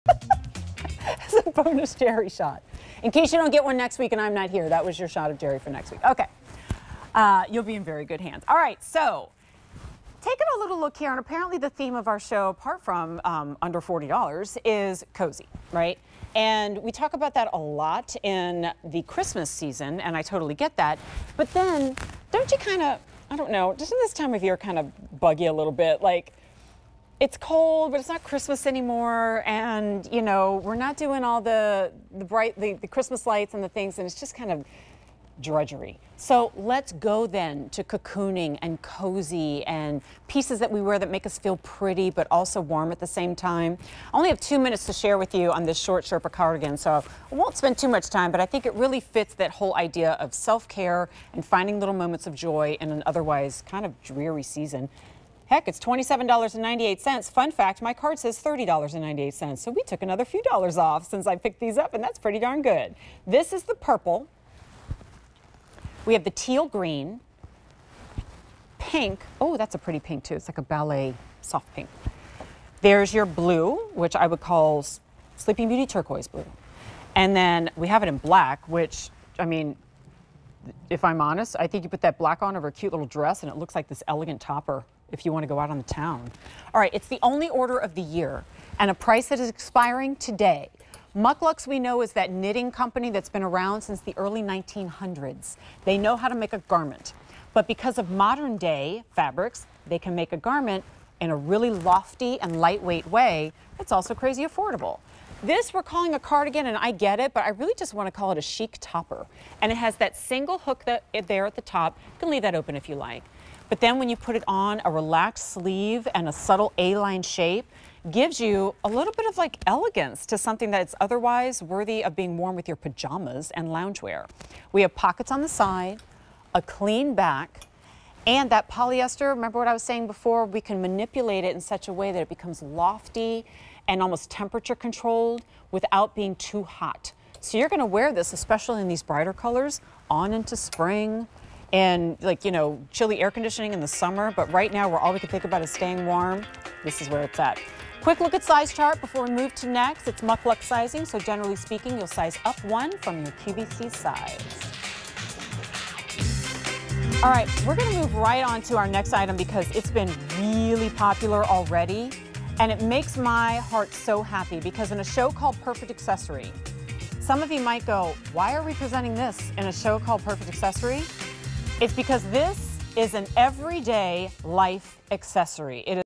MUK LUKS Short Sherpa Cardigan on QVC
MUK LUKS Short Sherpa Cardigan

Your winter wardrobe is now complete thanks to this sherpa cardigan that's a seriously soft barrier against the brisk weather. Wear it as a warm layer or a fashion statement with all the fuzzy feels! From MUK LUKS.